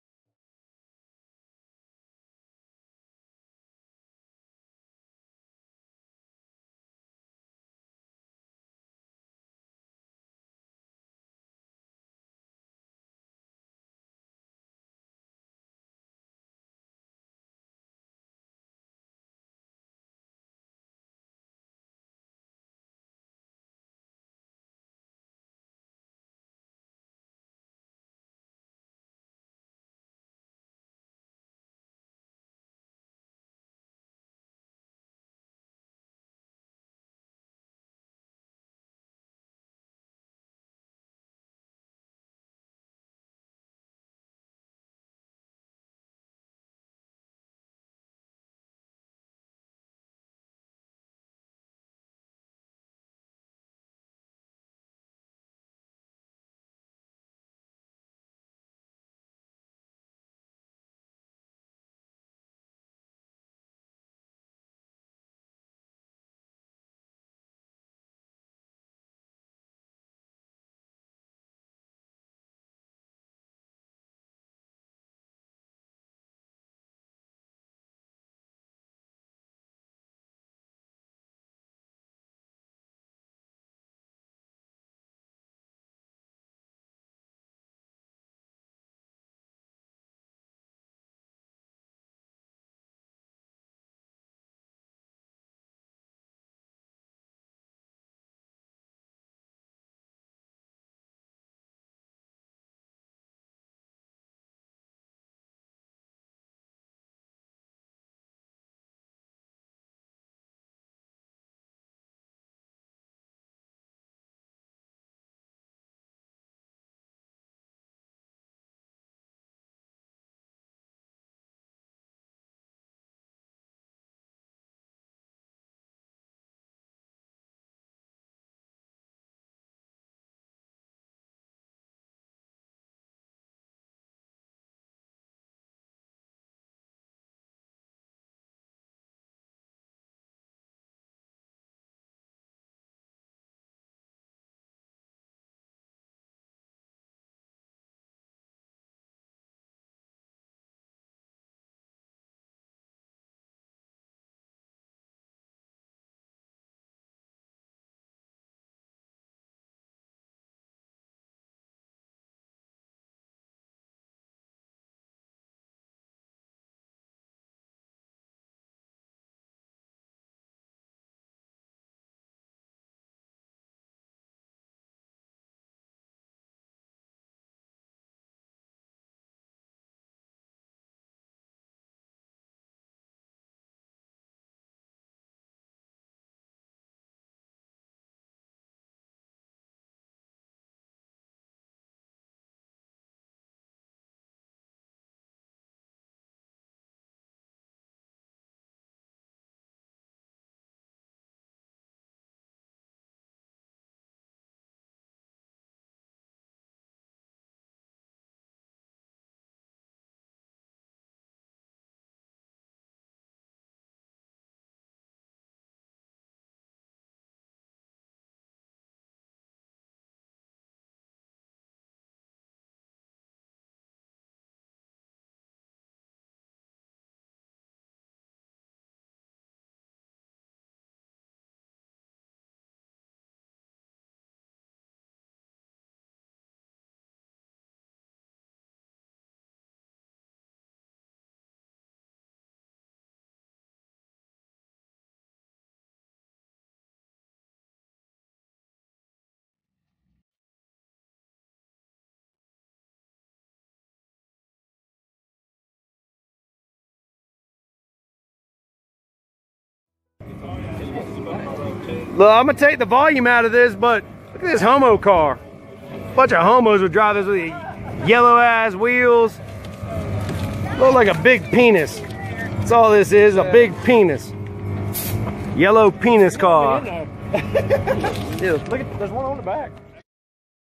HEAVY REBEL WEEKENDER 2018!!!! BADASS RAT RODS!! IT'S RAT ROD MONDAY!!!
Video I took from this past weekend at heavy rebel weekender located in Winston Salem NC